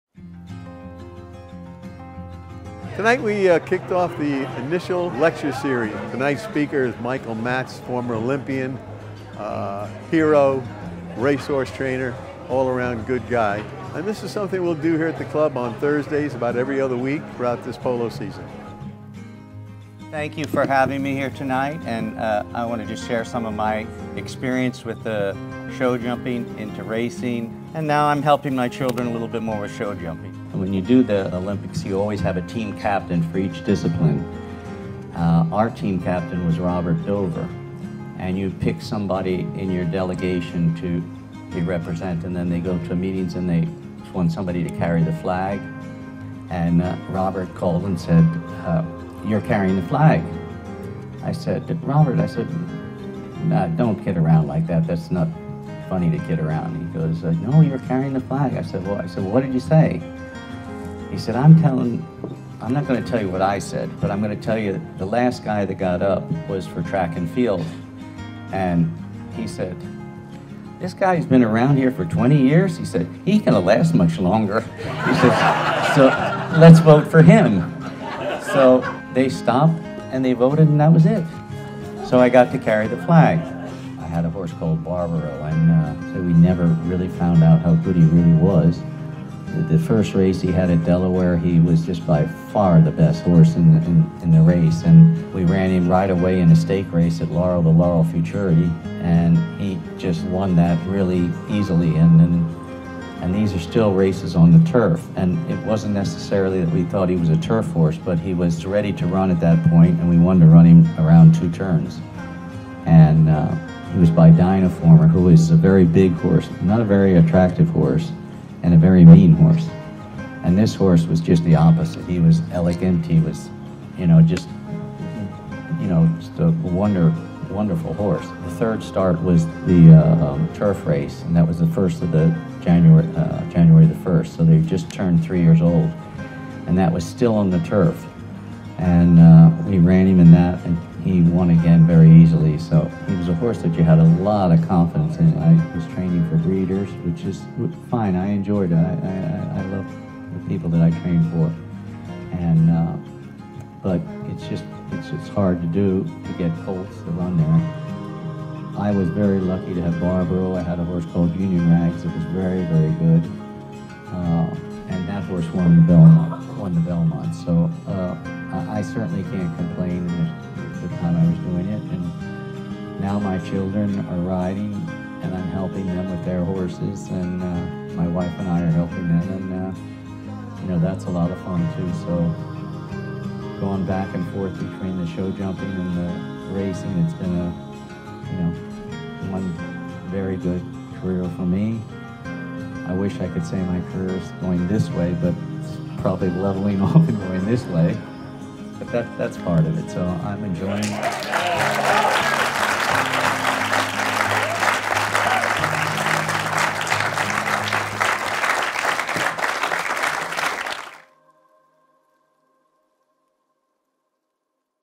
A night with Hall of Fame Horse Trainer Michael Matz
In this installment of NPC Speaker Series, we dive into the exciting life of hall of fame horse trainer Michael Matz, and his incredible story!